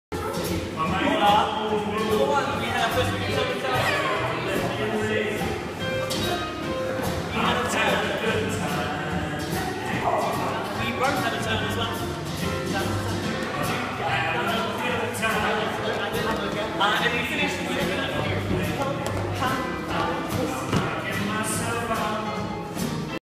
Swing Patrol Angel Level 1 Class Notes 13.05.14
Class notes for Swing Patrol Angel Level 1 Class on 13.05.14 at Candid Arts with Ben and Janet. Tandem Charleston
Facebook.com/swingpatrolangel